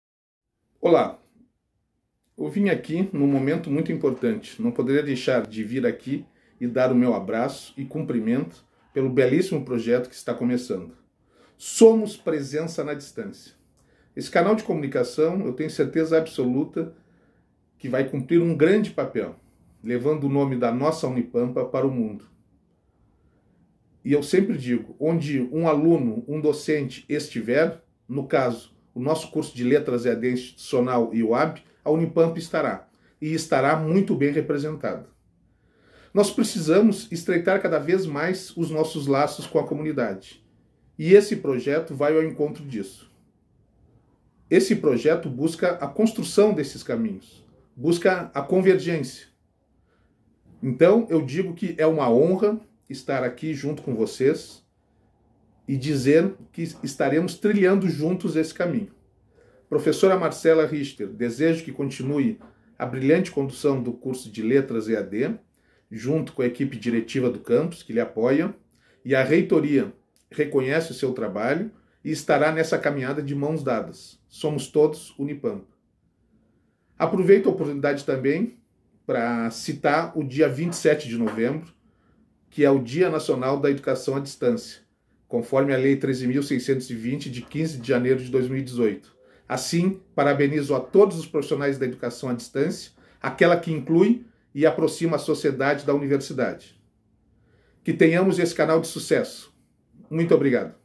Palavra do Reitor da UNIPAMPA
O Prof. Roberlaine Ribeiro Jorge, Reitor da Universidade Federal do Pampa - UNIPAMPA, deixa suas boas-vindas ao canal.